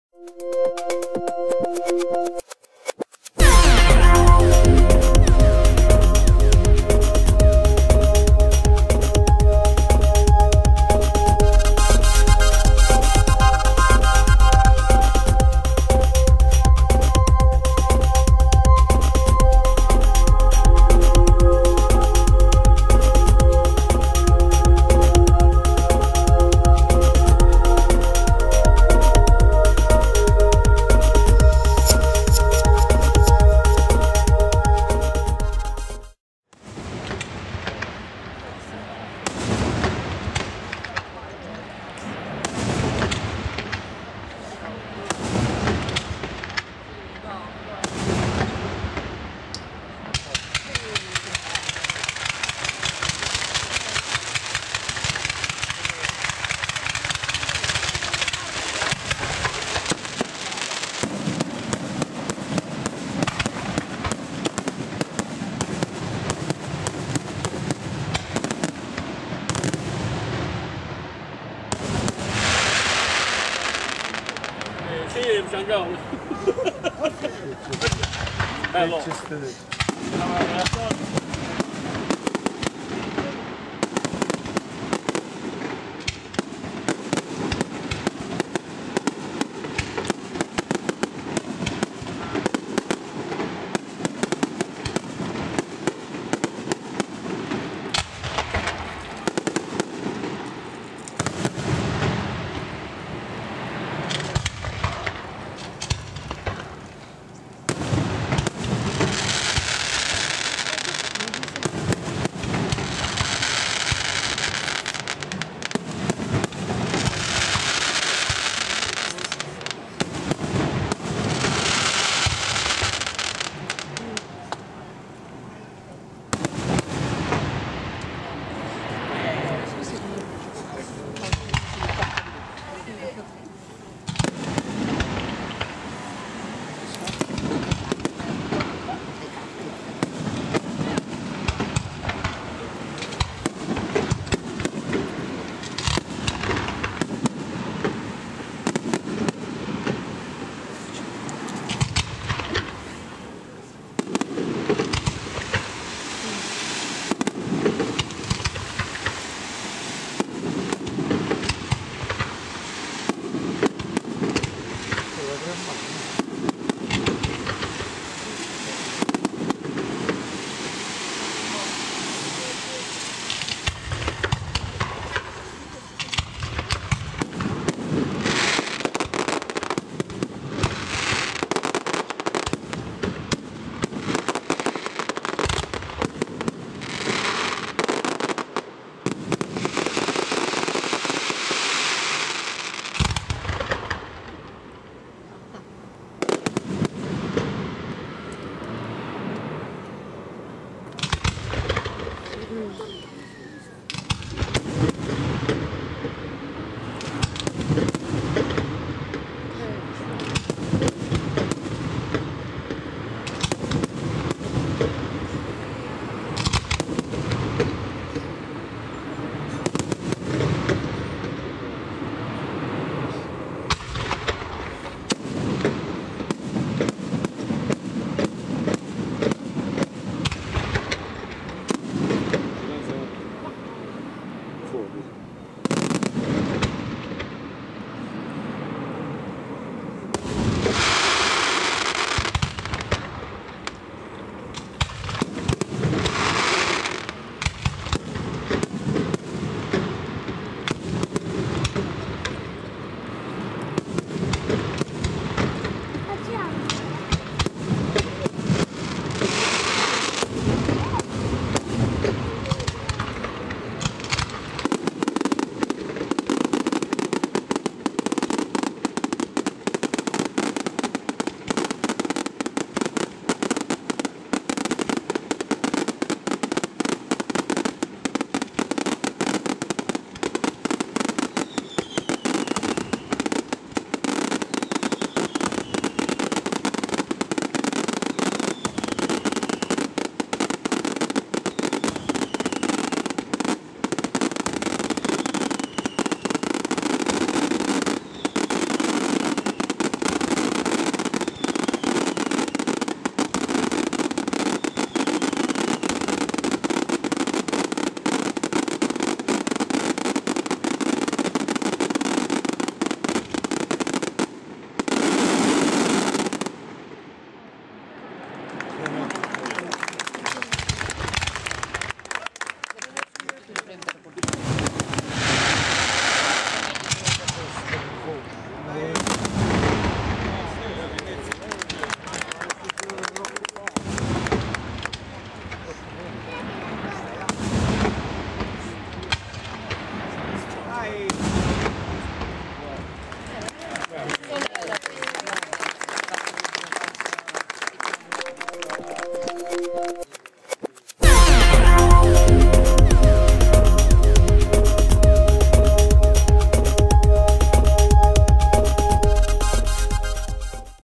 STORNARELLA (Fg) - I FUOCHI di S. PIO 2016 - PIROGIOCHI CHIARAPPA da S. Severo (Fg)
Stornarella (Fg) - 24 Settembre 2016
"I Fuochi di San Pio" - Festival di Fuochi d'Artificio

PIROGIOCHI CHIARAPPA da San Severo (Fg)